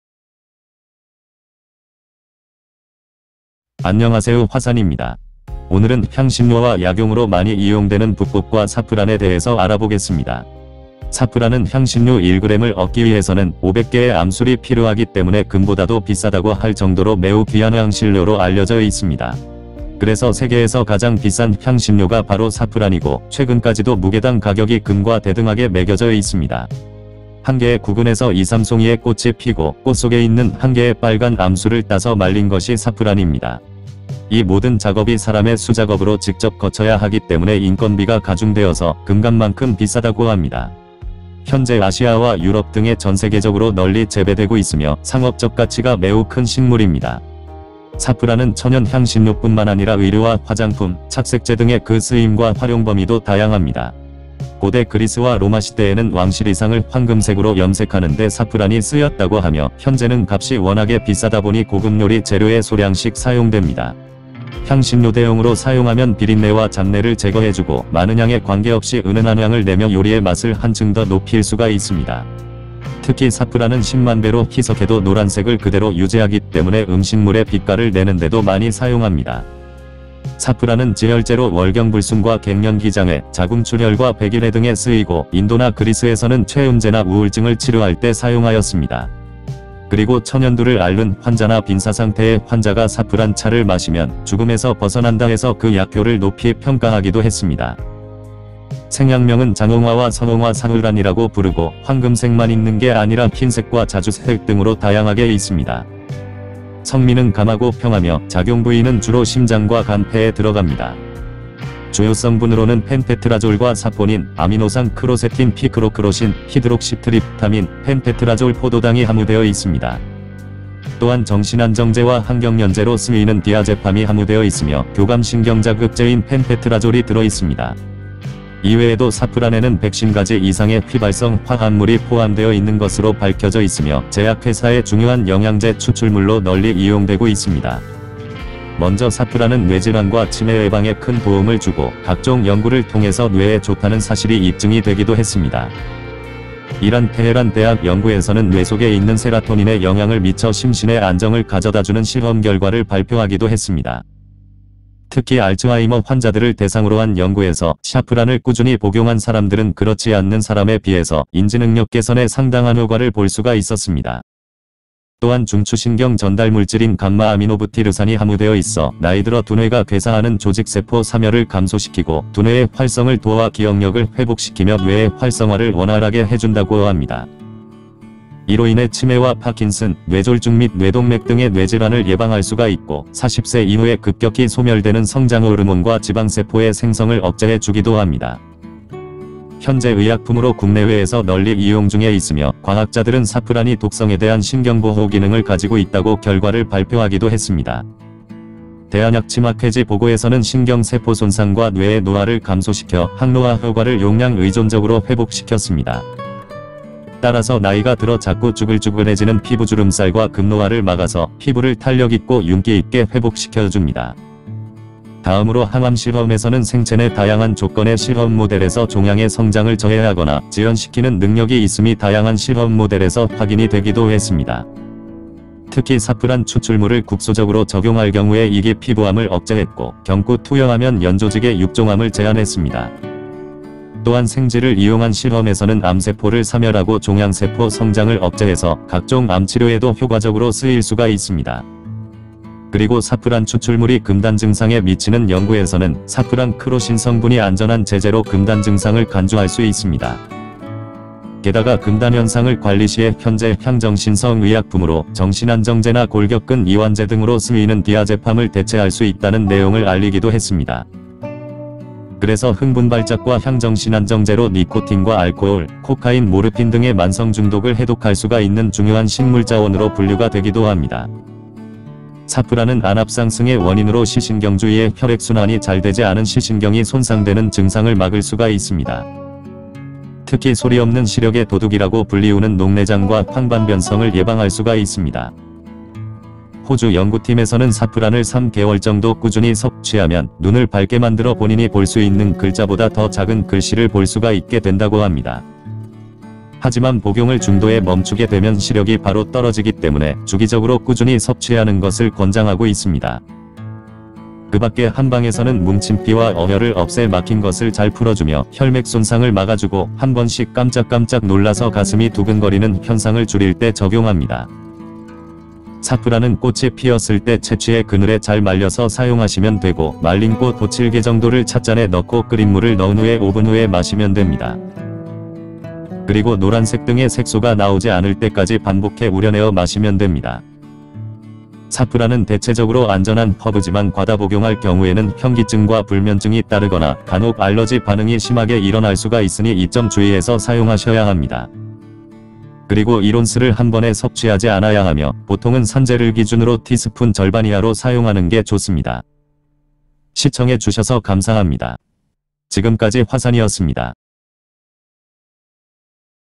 3개월만 먹어도 눈이 밝아져 작은 글씨까지 볼 수가 있고 치매와 뇌세포 괴사를 막아준다는 약초
녹내장, 황반변성, 치매, 알츠하이머, 뇌졸중, 피부암, 금단증상